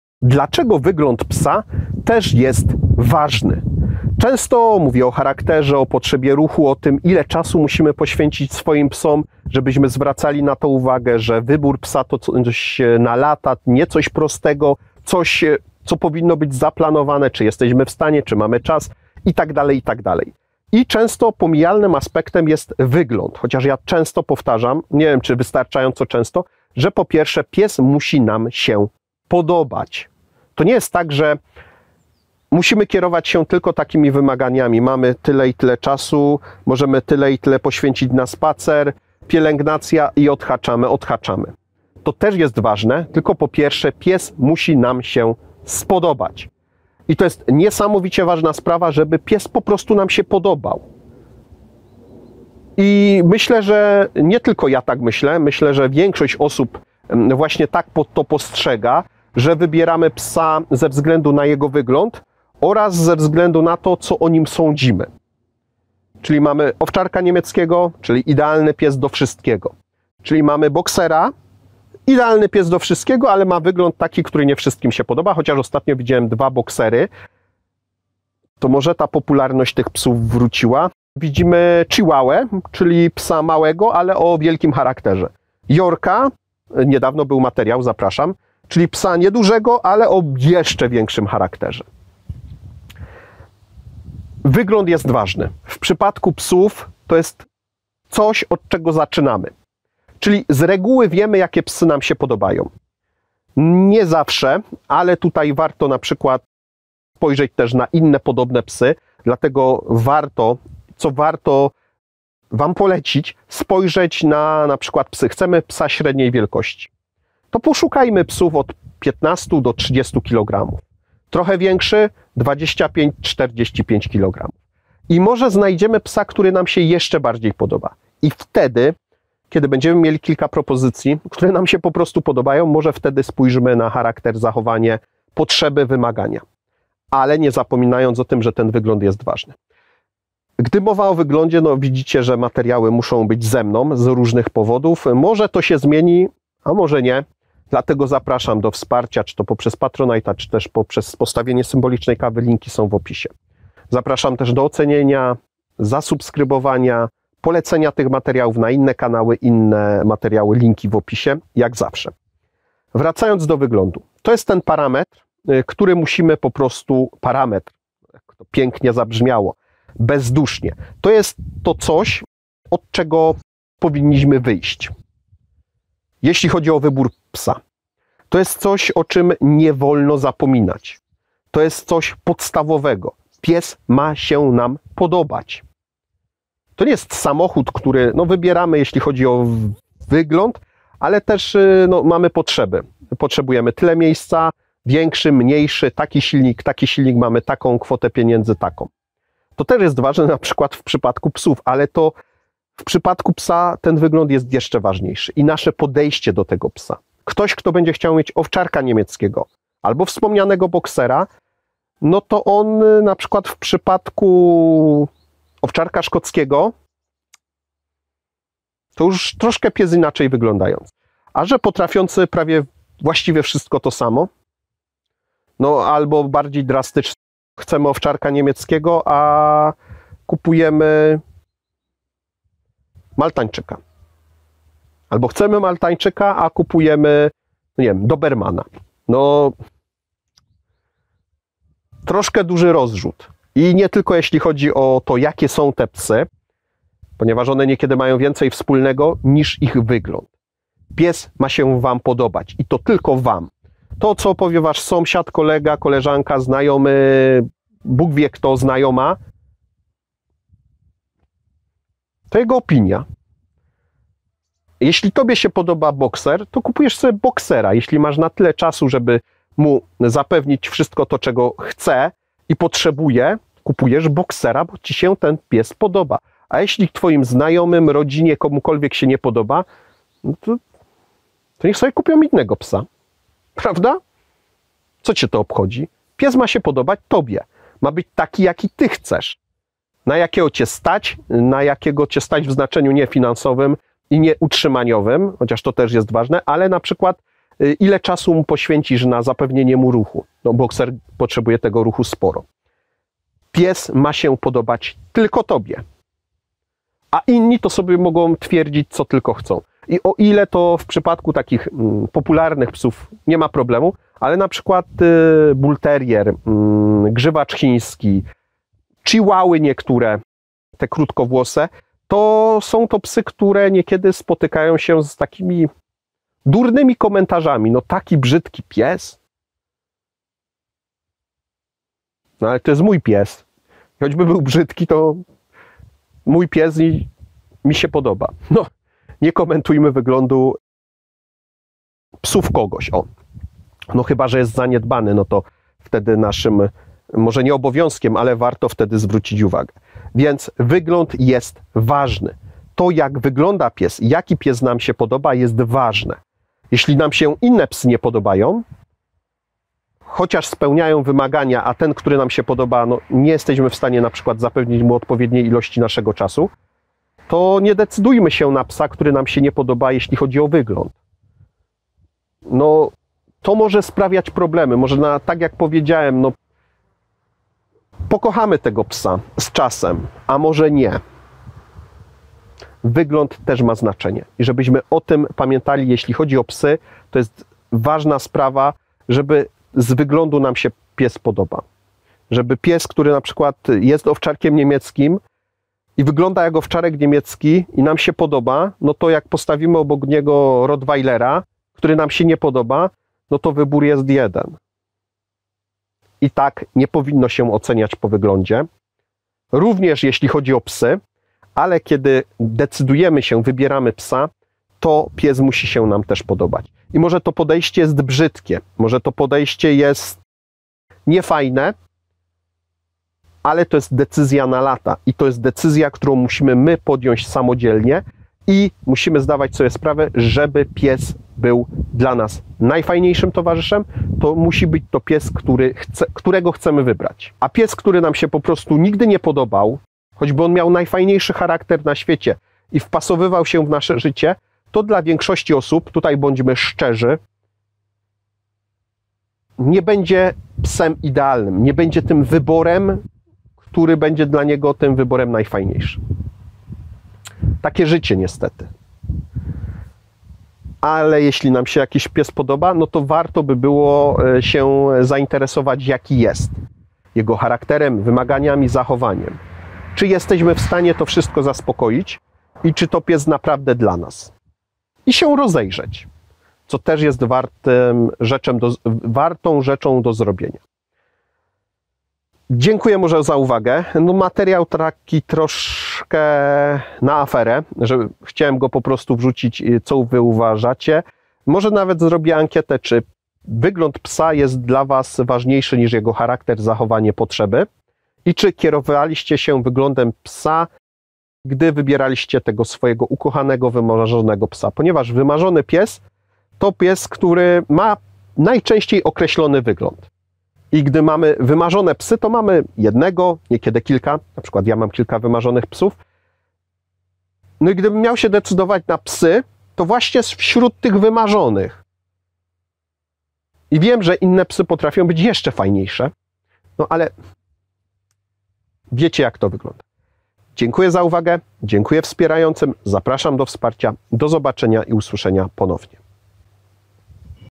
Dlaczego wygląd Psa też jest ważny
Choć nie powinniśmy na niego zwracać uwagi to jednak przecież on jest bardzo ważny gdy wybieramy dla siebie wymarzonego psa.